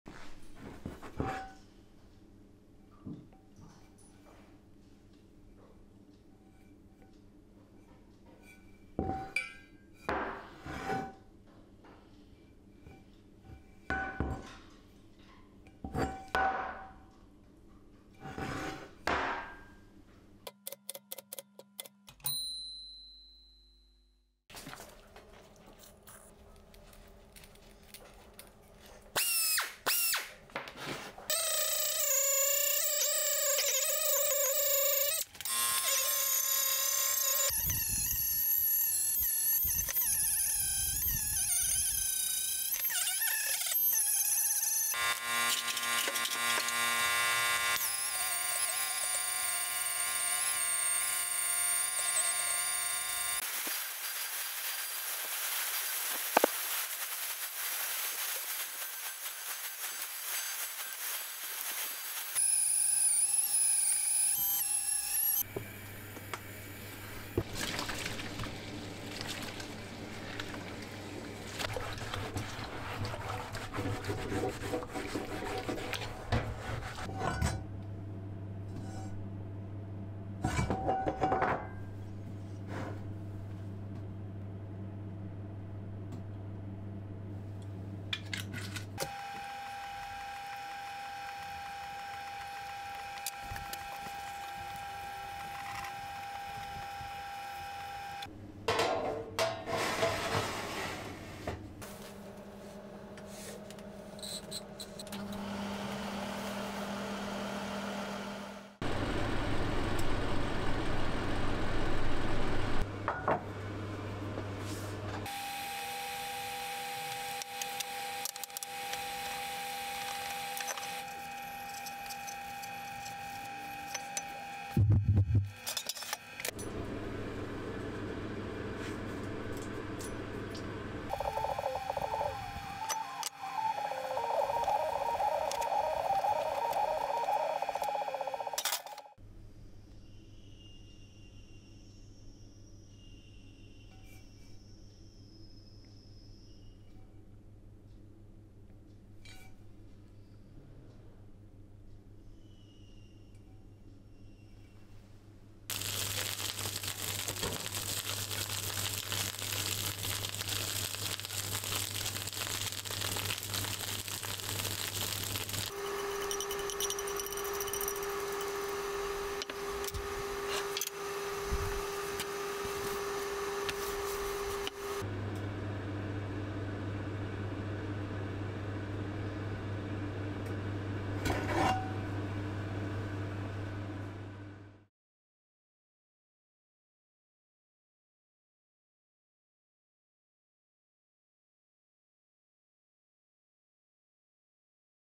Cast Iron Pan | How to bring an old pan back to life!
Flea market found... Love cast iron pans so I gave it a cleanup and resealed it... Have fun!

~My Partners~